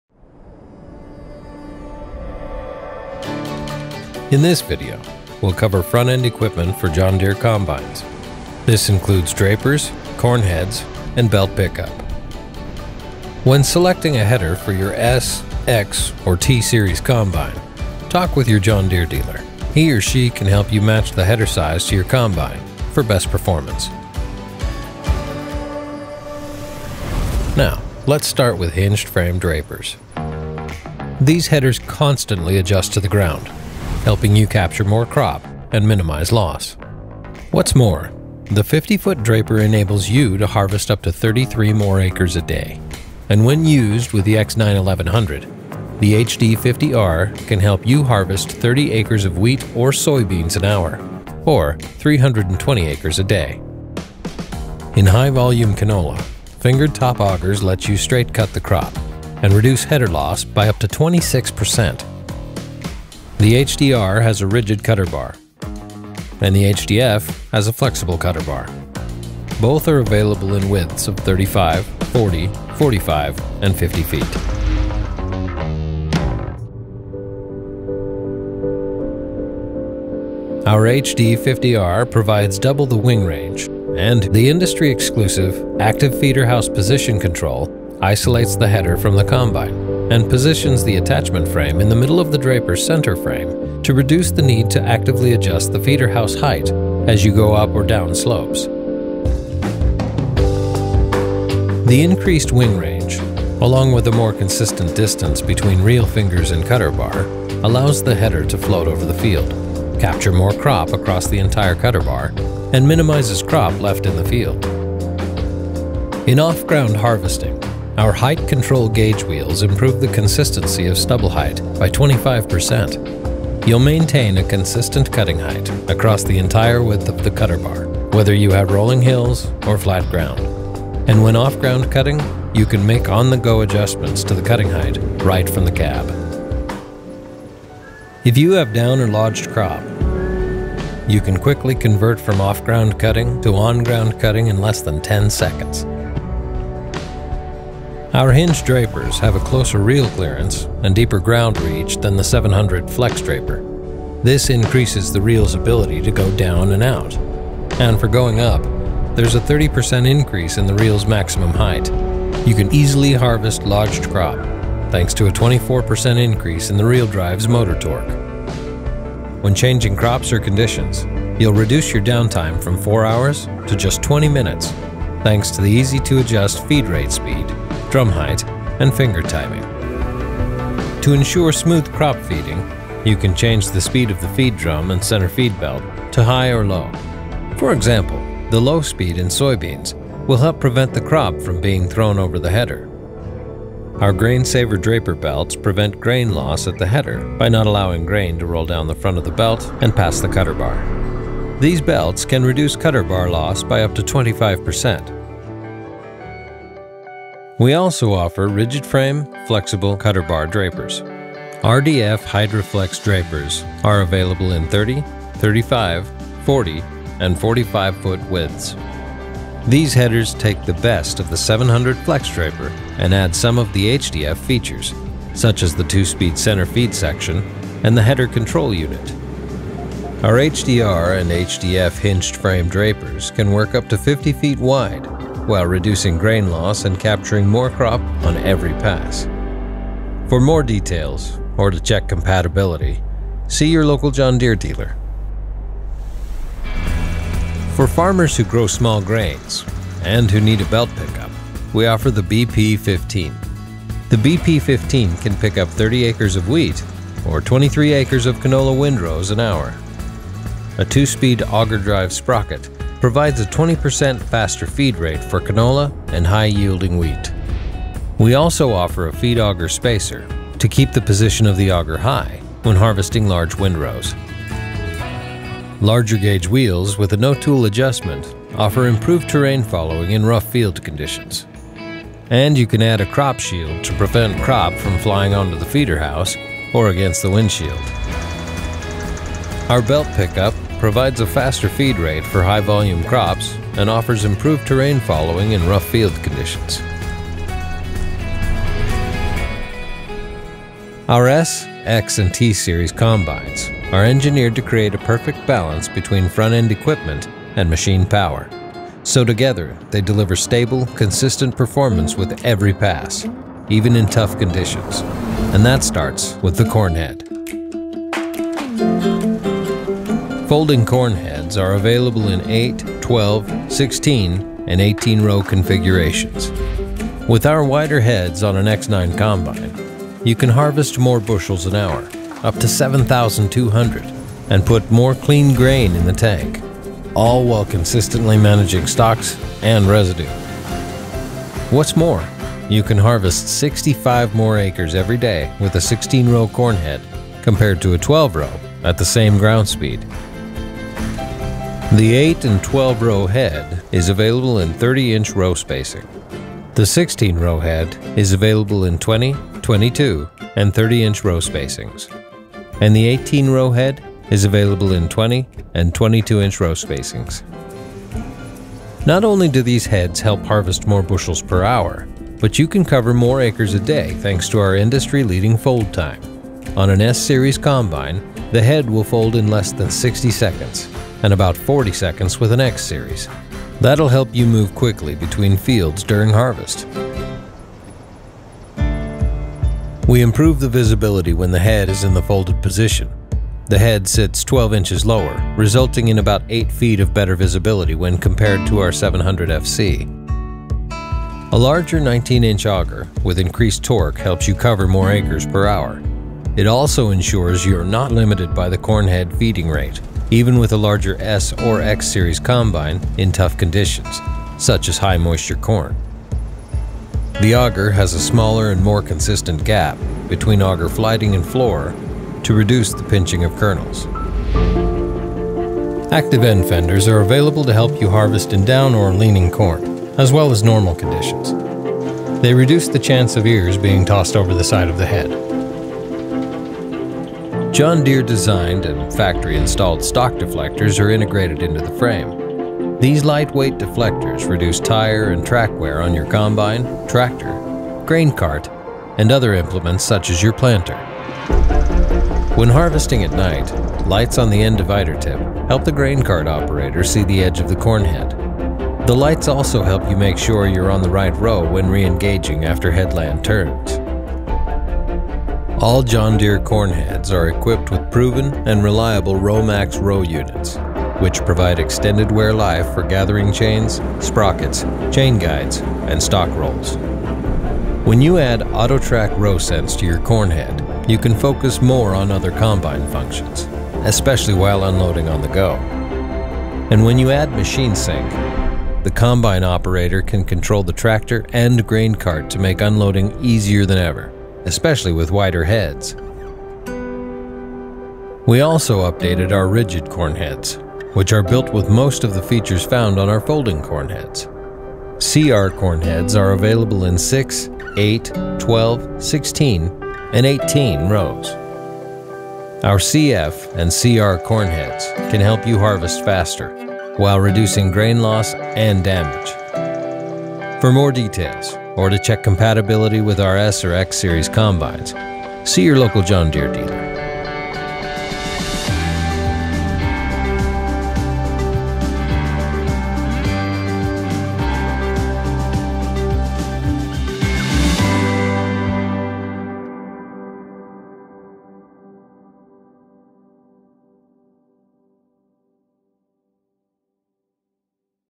Front End Equipment | John Deere X Series Combines
We’ve updated our full line of headers to match the increased harvesting capacity of our X Series Combines. Our drapers, corn heads, and belt pickup are also compatible with our S and T Series Combines. When selecting a header for your combine, talk with your John Deere dealer. He or she can help you match the header size to your combine for best performance.

Performance claims are based on internal testing, and on field and crop conditions at time of testing; individual results may vary. Preproduction models shown; specifications and design subject to change. HDF models not available in MY21.